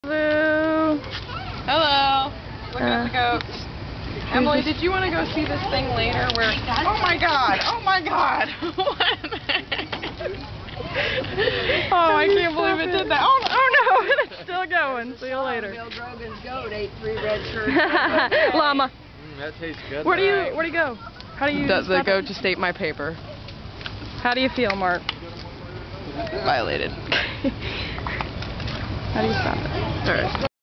The paper eating goat
A film of a goat eating my program at the Riverbanks zoo in South Carolina.